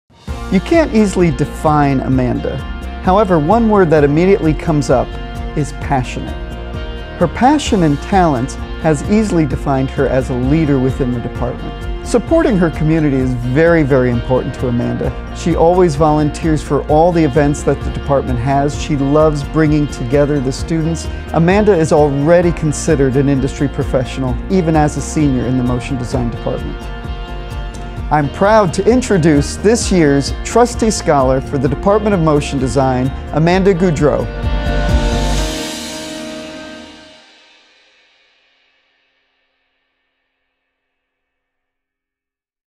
Amanda Godreau, Motion Design
Meet Amanda Godreau one of our MotionDesign 2021 Ringling Trustee Scholars. Push the boundaries of visual communications and technology with a Motion Design degree, learn more about how you can be like Amanda